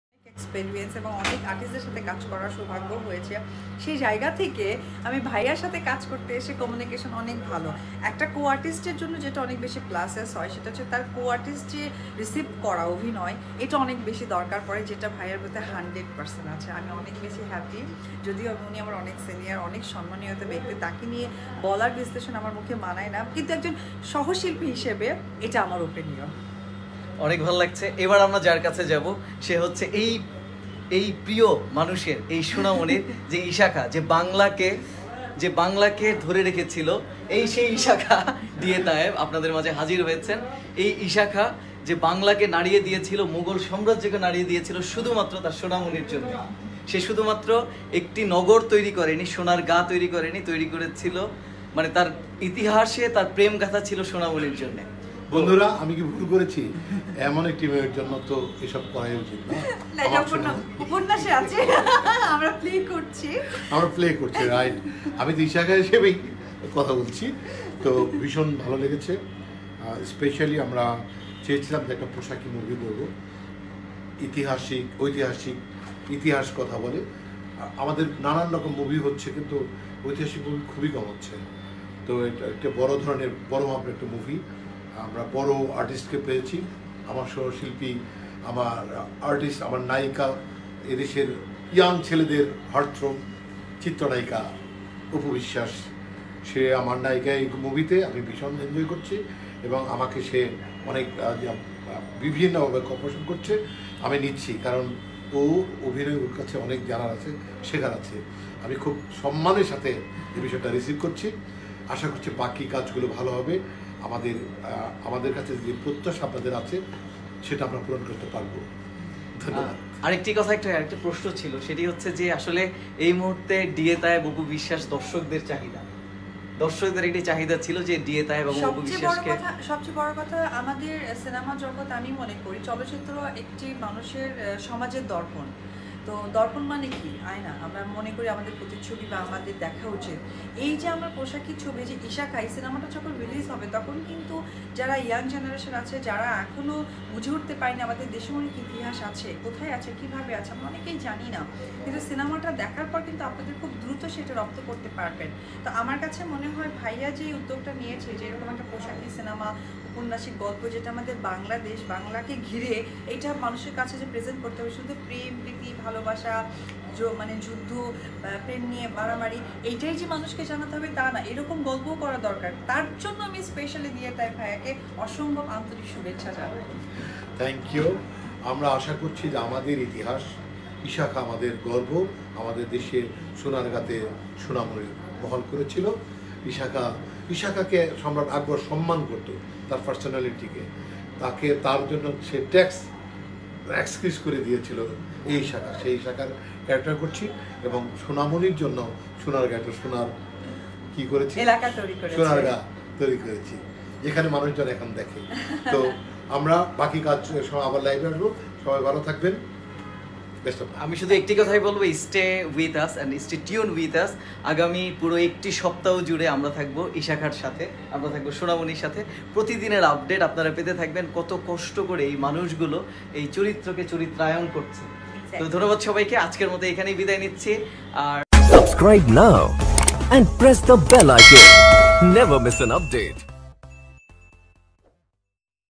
ইশাঁখা মুভির শুটিং স্পট থেকে অপু বিশ্বাস ও ডিএ তায়েব লাইভ|Apu Biswas|DA Tayeb Ishakha Movie Shottin
ইশাঁখা মুভির শুটিং স্পট থেকে অপু বিশ্বাস ও ডিএ তায়েব লাইভ|